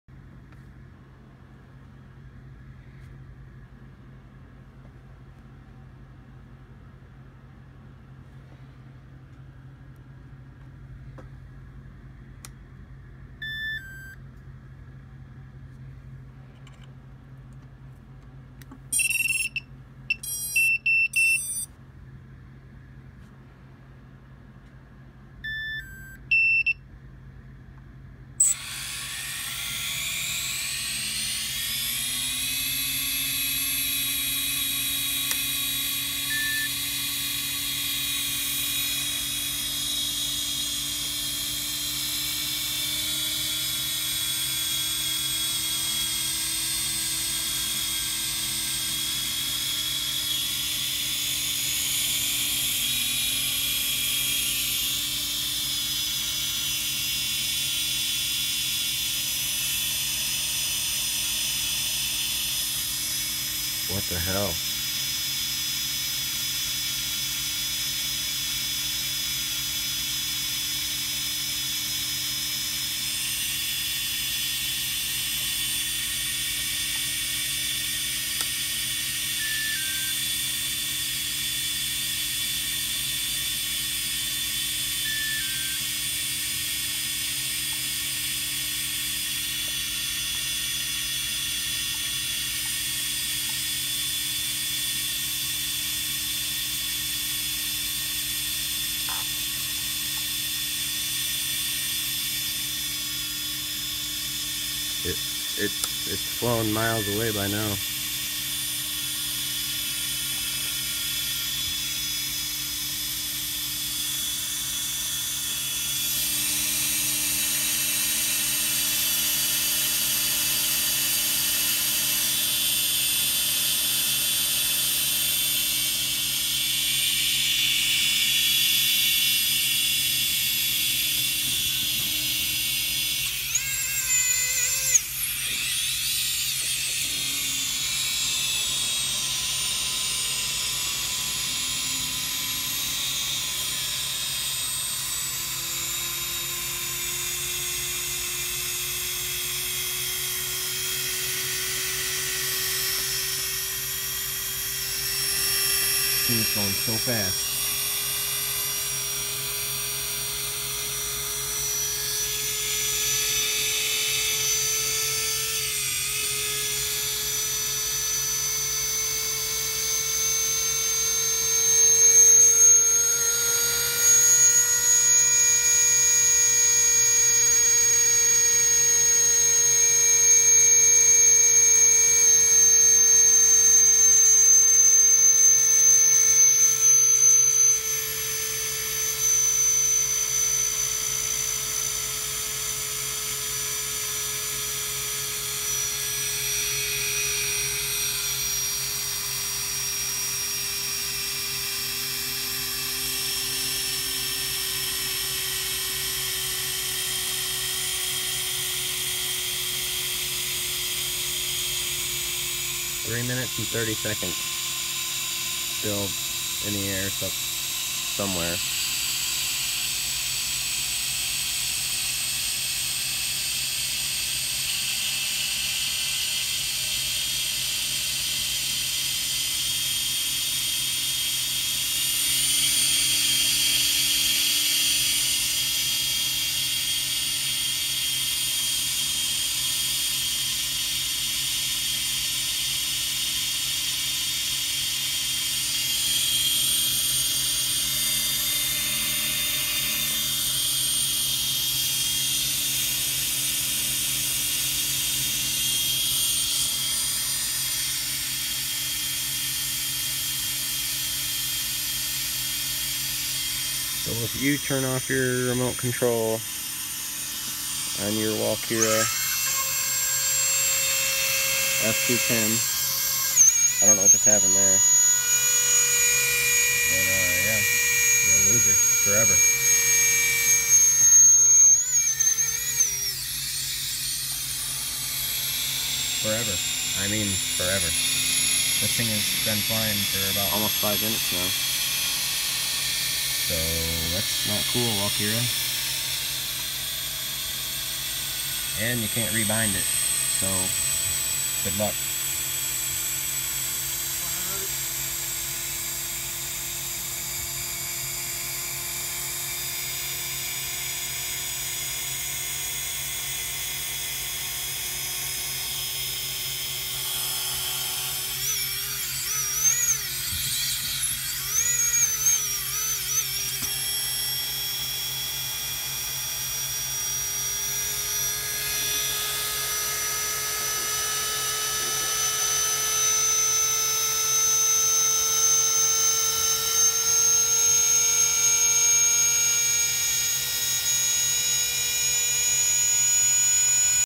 Walkera f210 fail safe issue
I hope the update with fix this issue, would be very misfortunate to loose this that easy.